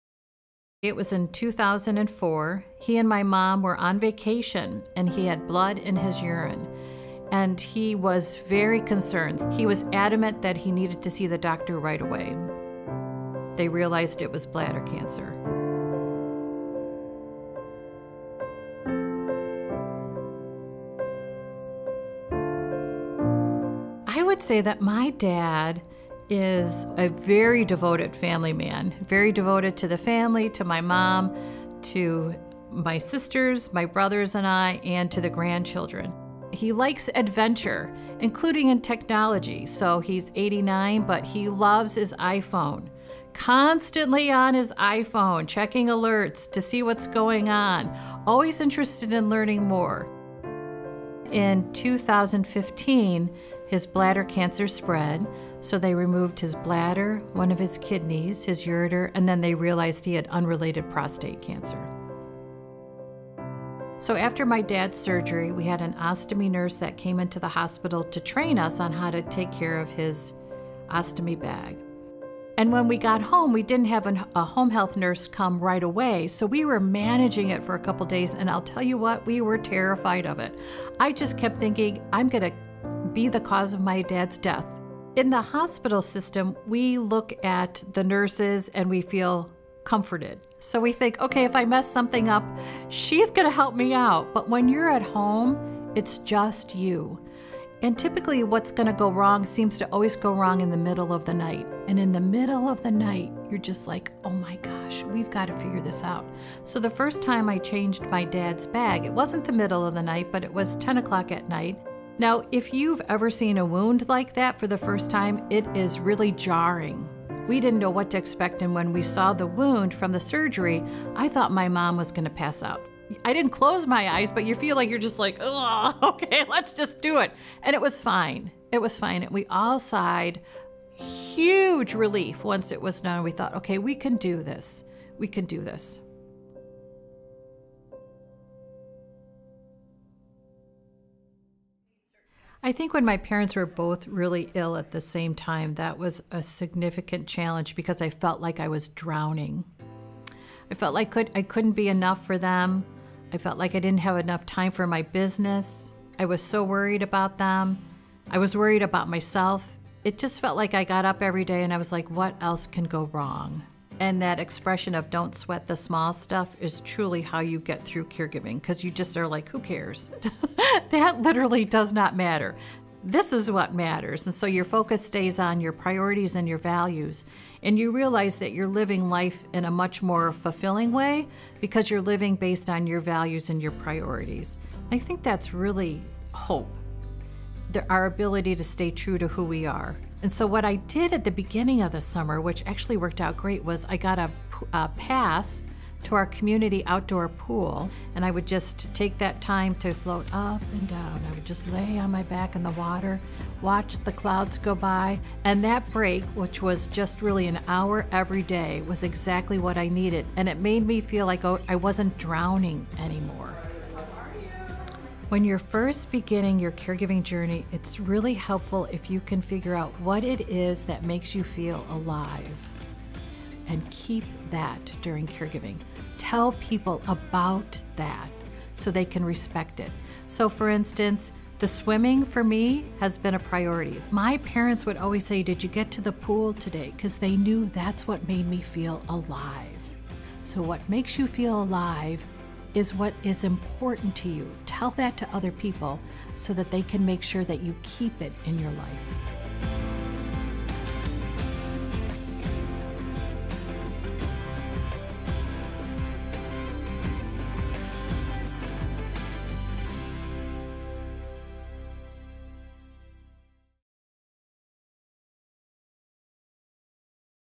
Caregiver Stories: Denise Brown
Denise details her experience providing care to her father with bladder cancer. She also provided care to her mother at the time who had various medical conditions. Listen to her journey and find out how she found time to center herself and renew her energy for caregiving.